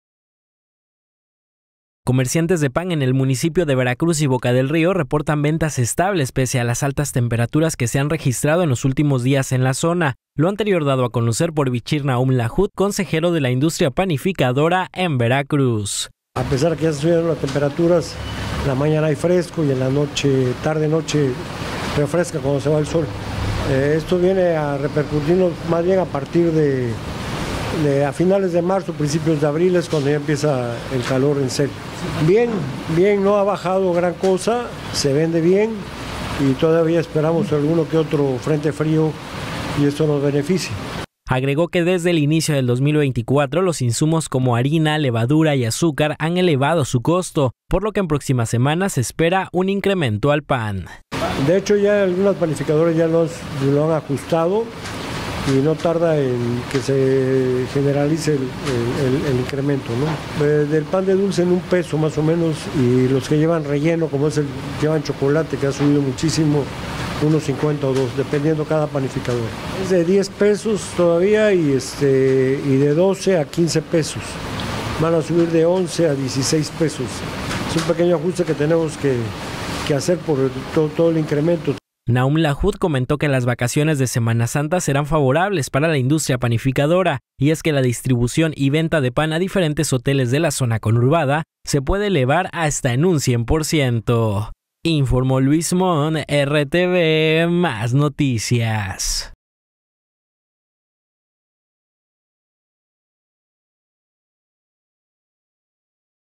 Se estima un próximo aumento en precio del pan de dulce en Veracruz
Veracruz, Ver.- Pese a las altas temperaturas que se han registrado en los últimos días en la zona conurbada, la venta de pan se ha mantenido estable, así lo comentó Bichir Nahum Lajud, consejero de la industria panificadora en Veracruz.

Agregó que en los próximos días se espera un incremento en el precio del pan dulce, el cual irá desde un peso y hasta los dos pesos.